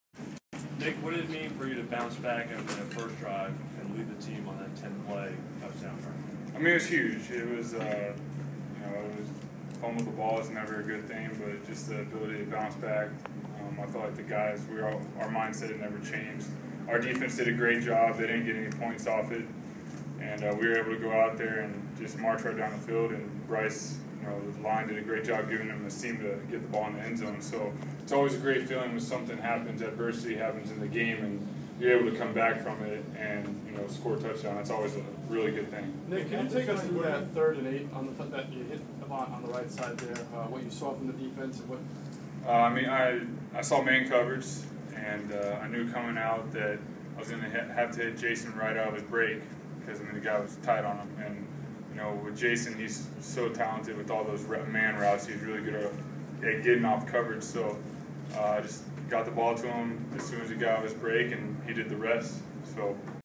Eagles Quarterback Nick Foles
Eagles quarterback Nick Foles talk about his performance on Friday night, including the 10-play drive he led for a touchdown. By Ed Kracz.